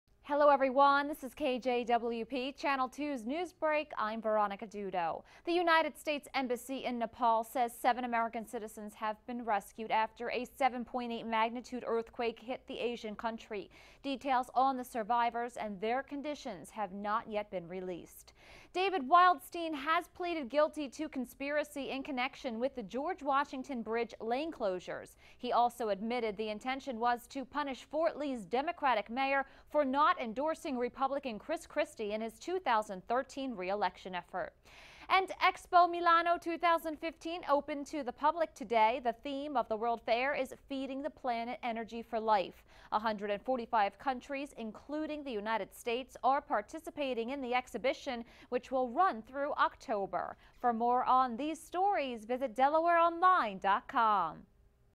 KJWP-2 Wilmington | Philadelphia News 5.1.15
American citizens rescued from Nepal, conspiracy charges in the George Washington Bridge lane closure scandal, and Expo Milano 2015 opens to the public.